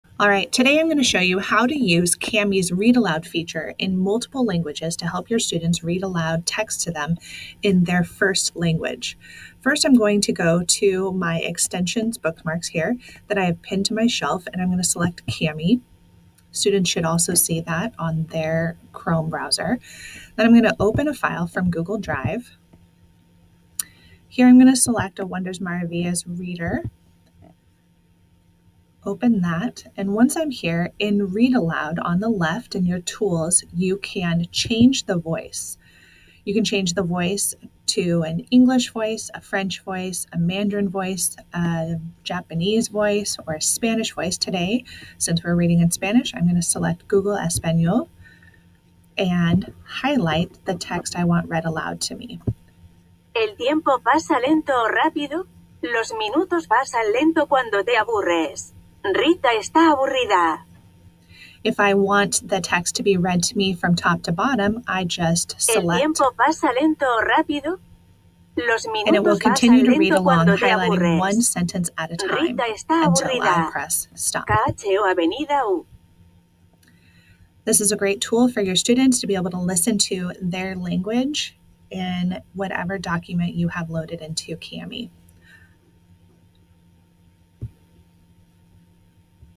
Kami Read Aloud Feature
Did you know that Kami will read to your students in Spanish and many other languages?!

Load your documents in Kami (PDFs, Google Docs, Google Slides, etc.) or open from Google Drive within Kami and choose the Read Aloud feature.

Students can then change the voice to read with Google Español, Google Japanese, or even Google Mandarin and alter the speed at which it reads aloud.  You can select a sentence, a paragraph, or set the Read Aloud feature to read the document from top to bottom!